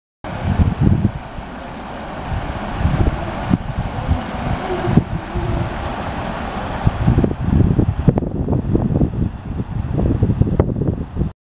流れと音と形の観察 （超音波システム研究所 ultrasonic-labo）
川（八王子市　湯殿川）の流れを観察しています

注：
くりかえし
　超音波と
　流体の変化（流れ、渦、波・・）を
　観察して

1年ぐらい経過してくると
　渦の動きが見えてきます
　そこから

音響流
一般概念

音響流は、
　または
　音場内の

波の問題は